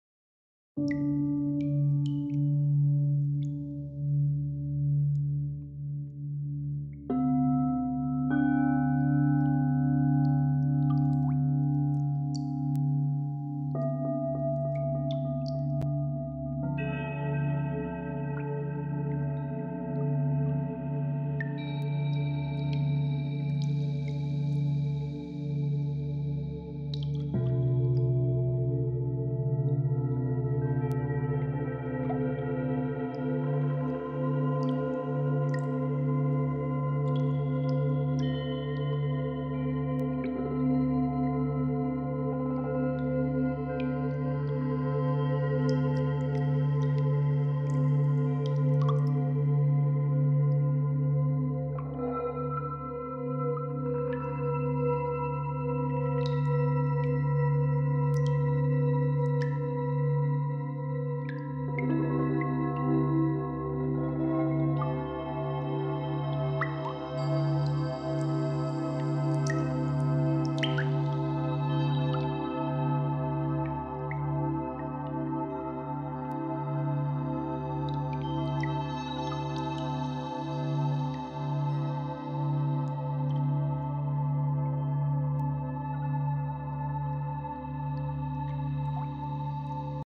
New 432 Hz CHAKRA BOWLS & DRONES TEMPLATE coming this friday!
The template is out now :)

I have some issues with the login of my online shop so for now you can get the template here:

That's why I love to combine organic instruments with the processing power of a computer 😍

A single set of Crystal Bowls turns into an ambient symphony.

On Friday I will release my new 432 Hz CHAKRA BOWLS & DRONES TEMPLATE for Ableton.

Its perfect to accompany and expand your existing sound healing setup or a very practical mobile option for anyone working with individuals or groups with sound in a healing context.